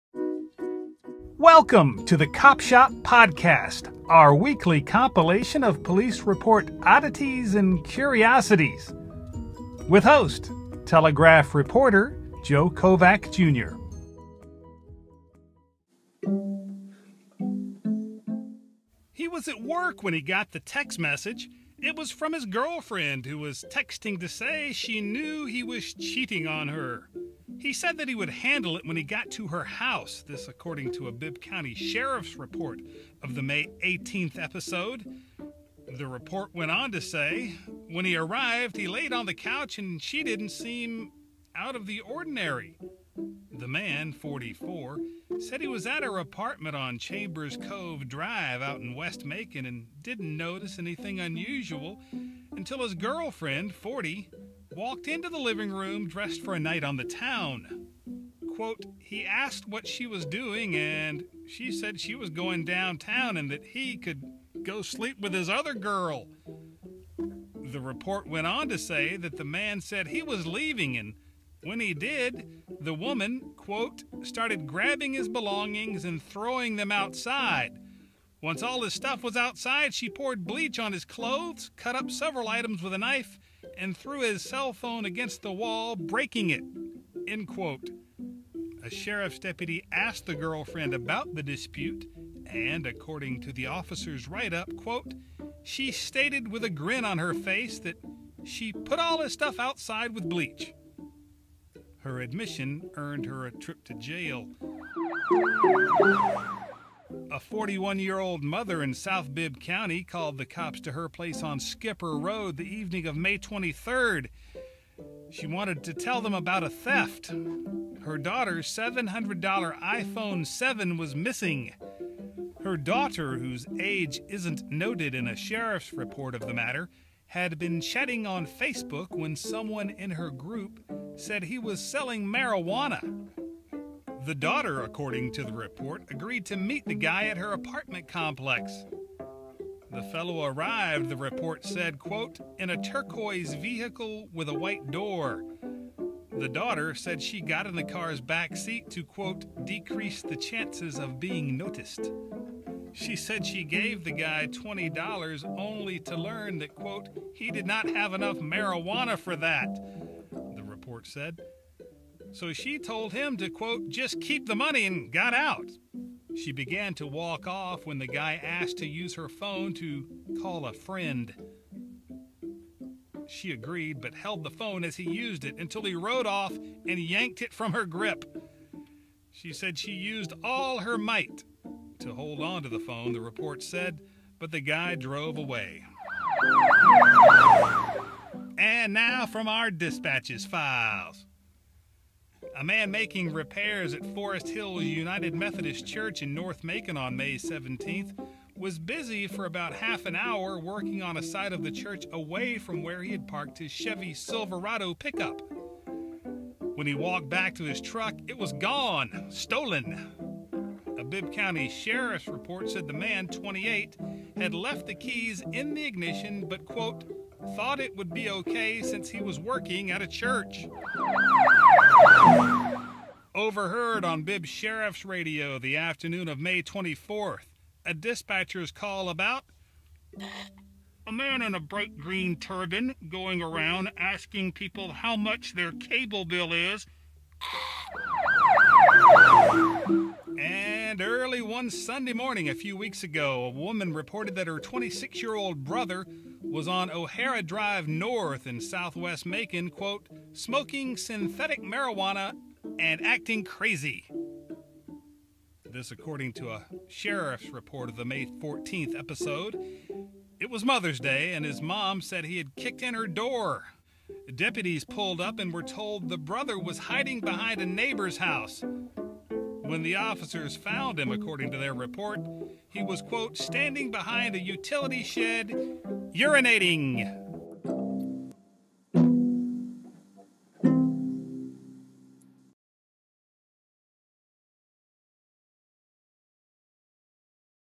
Woman gets even with 'cheating' beau, gets arrested
This episode of our audio journey into Macon-area police report oddities includes items about a "cheating" boyfriend; a stolen iPhone 7 taken in a marijuana deal; and the always amusing sounds of things overheard on our police scanner.